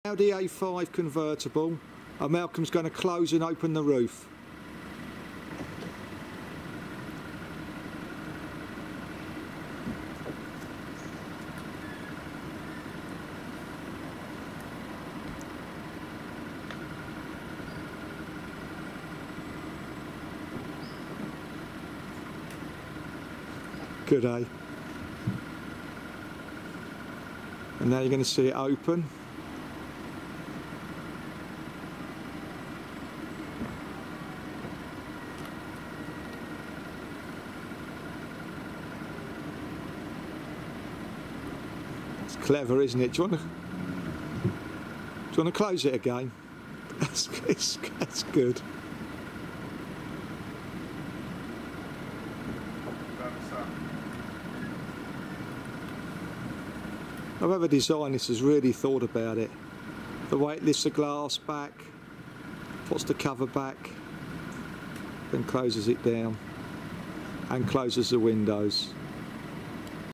Audi A5 Convertible Electric roof operation.
Showing the A5 Convertible electric roof in operation. A really well thought out piece of kit.